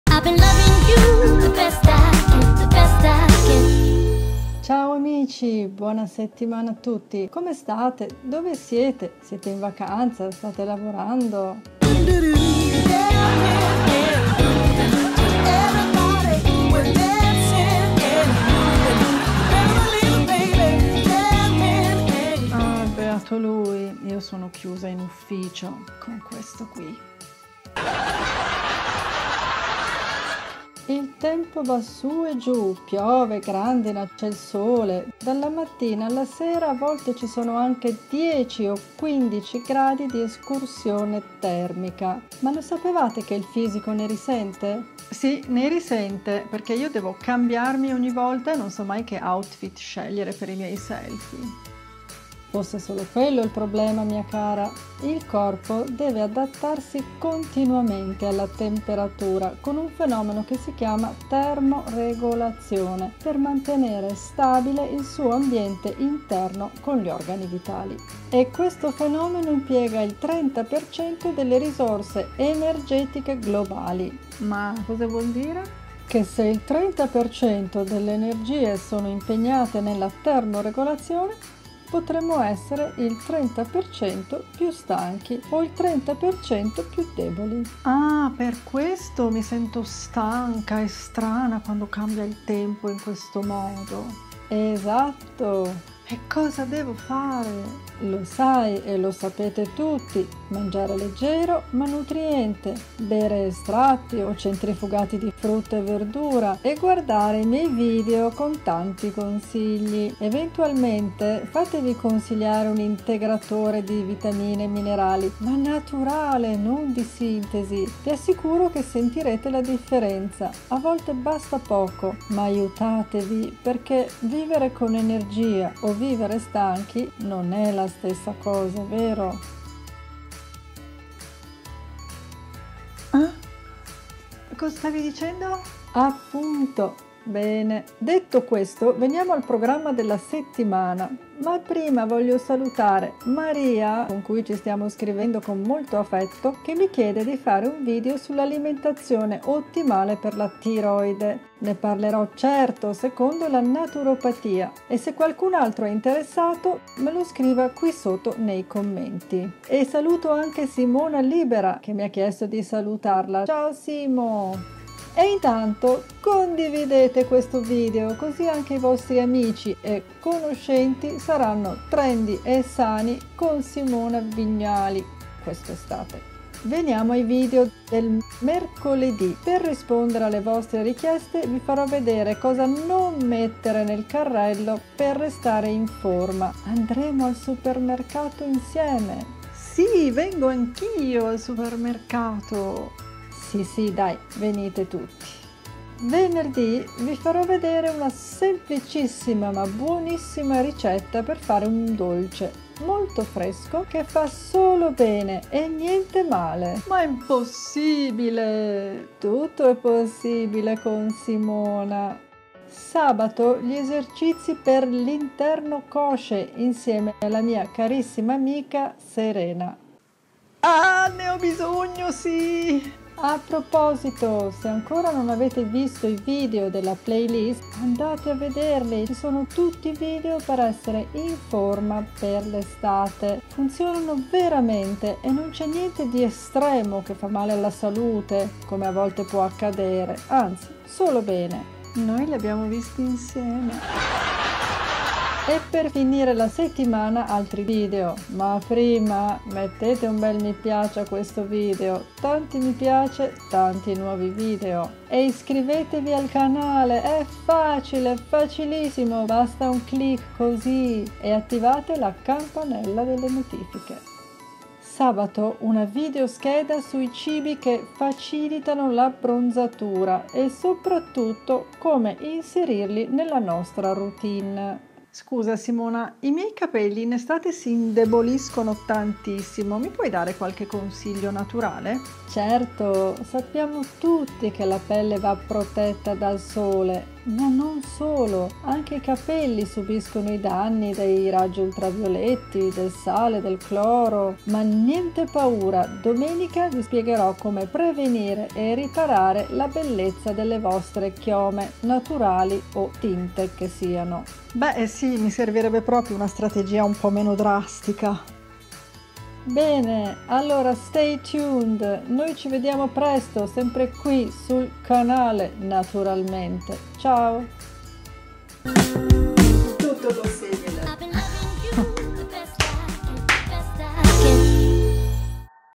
INIZIAMO BENE CON SIMONA #21: cosa MANGIARE in ESTATE + CURA CAPELLI NATURALE
Ciao amici!

Con queste temperature pazzerelle - caldo, freddo, sole, pioggia, grandine... - e con questo caldo, bisogna fare attenzione a non ammalarsi. Per sostenere il fisico basta seguire i miei consigli!

Ve ne dico alcuni in questo video, poi guardate gli altri miei video. Questa settimana vediamo cosa non comprare per dimagrire, come proteggere i capelli e tanto altro...

Nota bene

Simona Vignali